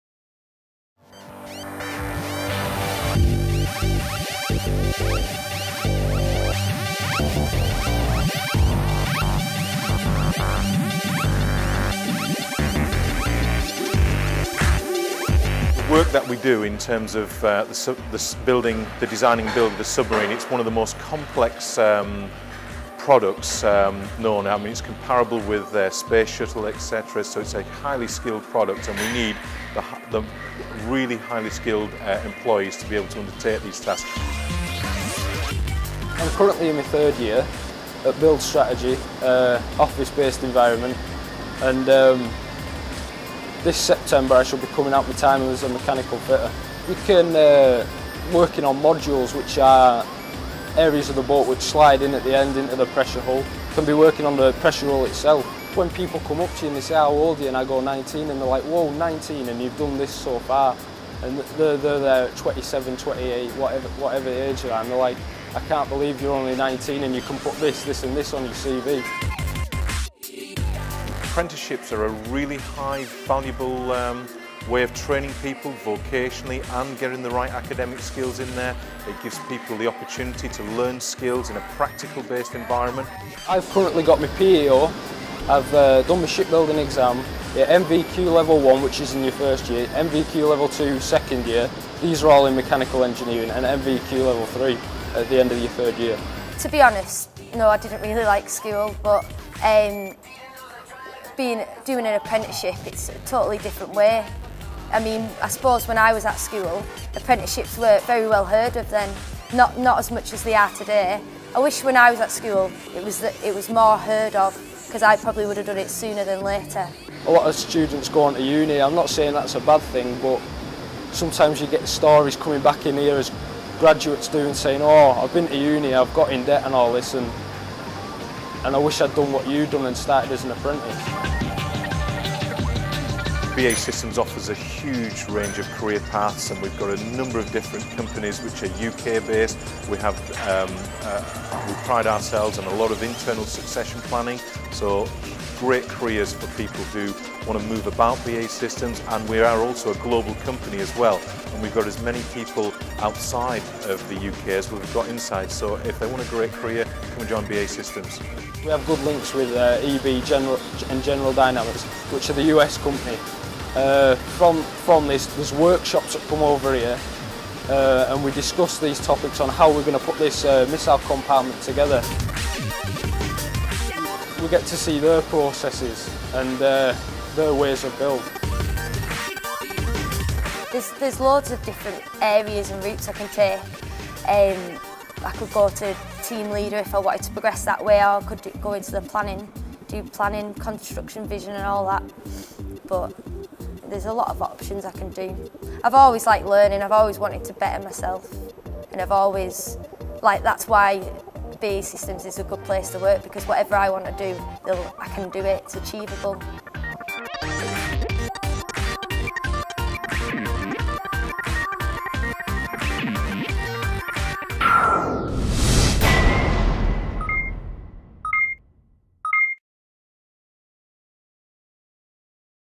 BAE Systems Apprenticeship Programme
How the National Employer Service has worked with BAE Systems to support and develop their Apprenticeship programme.